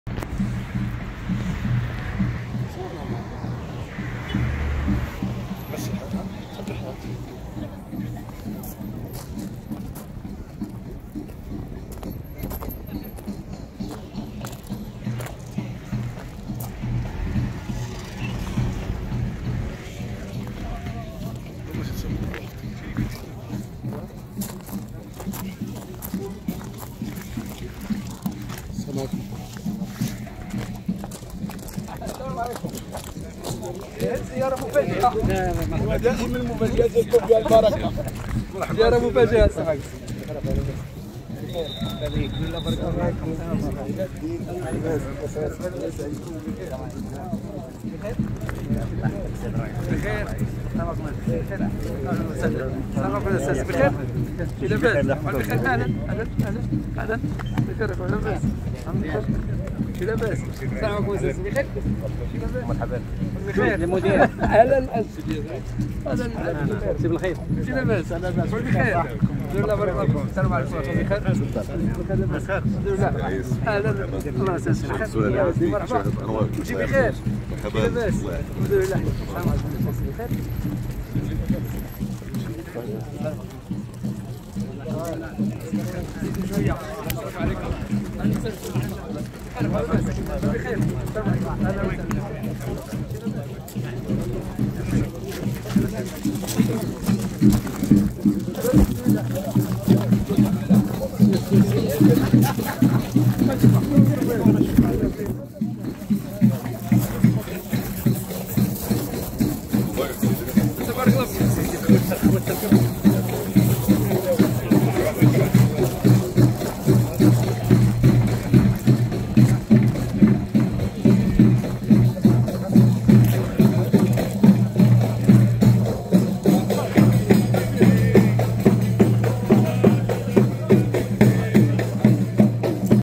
شاهد زيارة مفاجئة لرئيس الحكومة لمهنيي الفخار و الزليج بفاس
Video from Ahmed Bekkali Namita